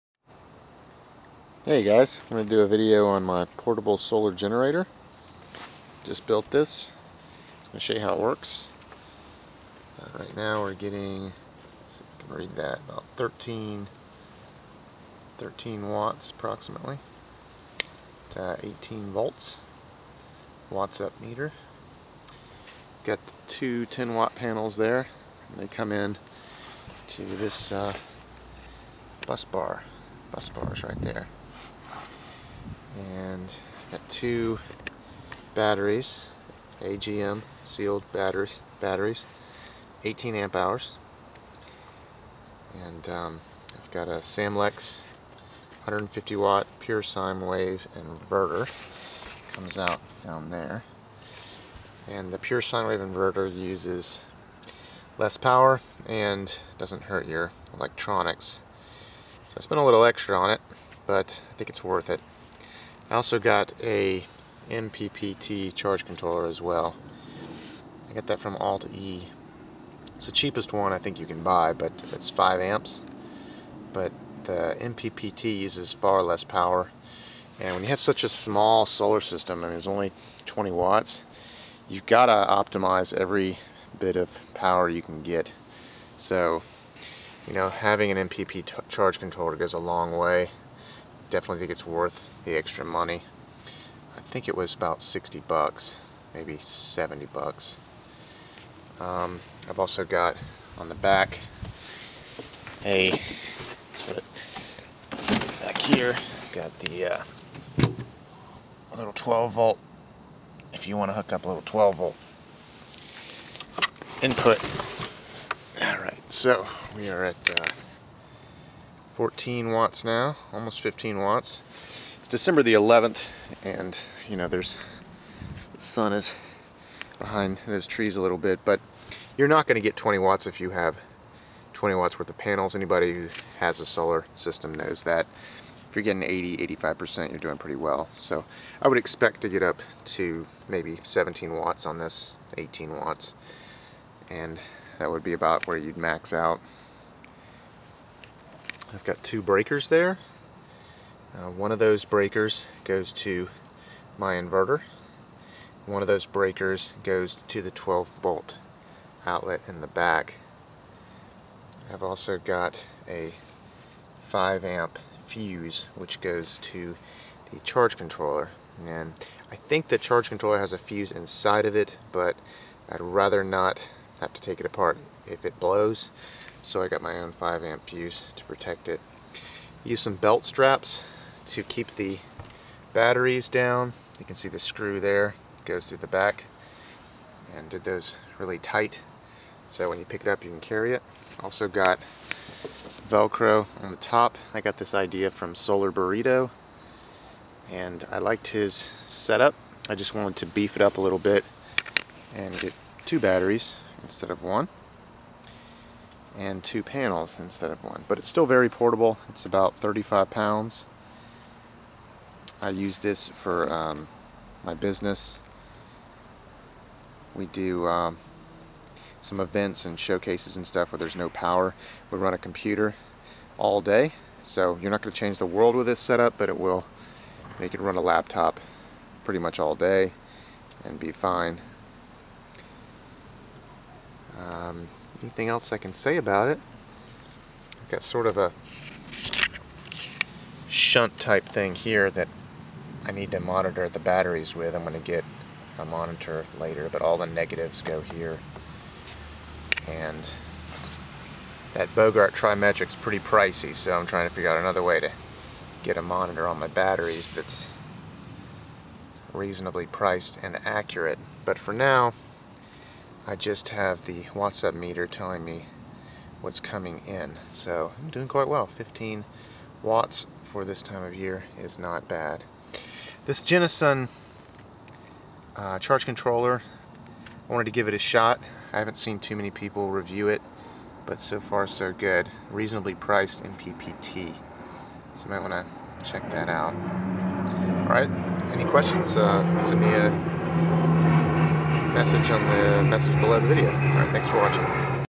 Portable Solar Generator (20 watt)
Video of a Portable Solar Generator I just made.  It is about 35 lbs and is made from a Ridgid hard case.  It has two 10 watt panels, two 18 Amp hour batteries, a Samlex Pure Sine 150 watt inverter, a Genesun 5 amp charge controller and a 12 volt input.